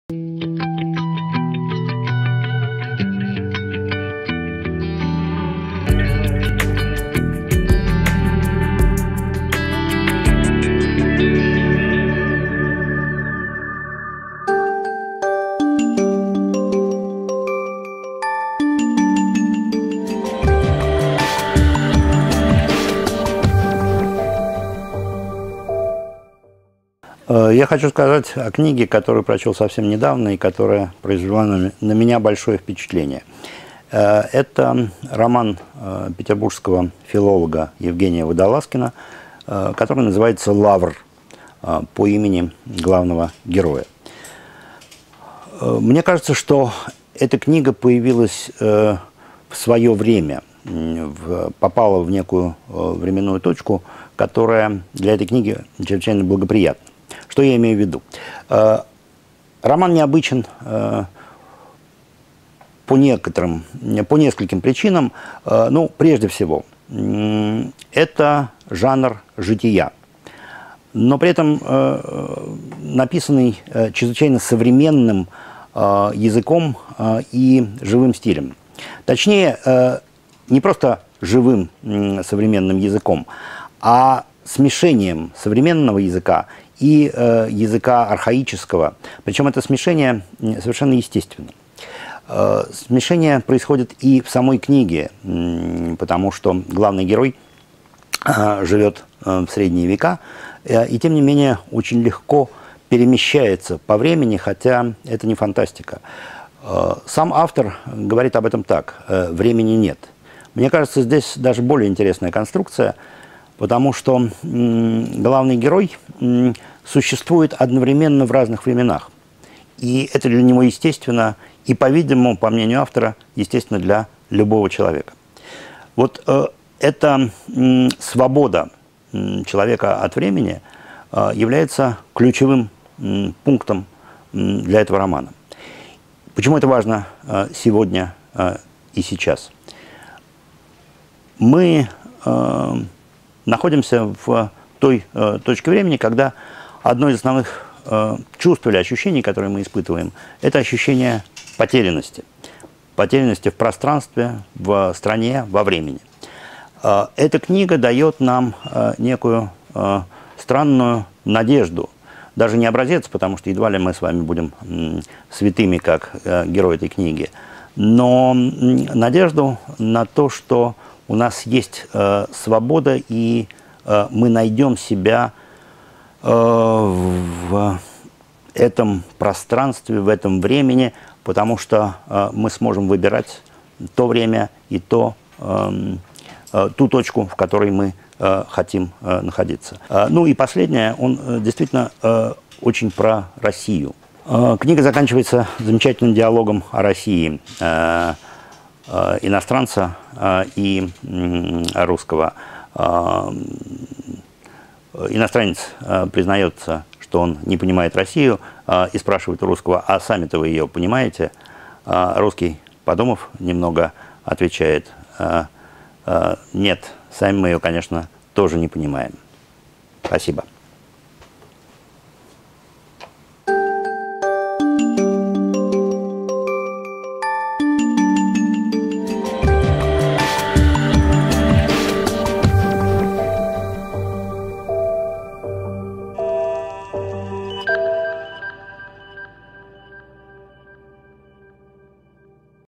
Книжная среда - Е. Водолазкин "Лавр"
В этом выпуске Максим Кронгауз рассказывает о книге Евгения Водолазкина "Лавр". Роман Евгения Водолазкина «Лавр» формально о Средневековье. Однако внутри этой формы время течет нелинейно, проступая то современностью, то вечностью.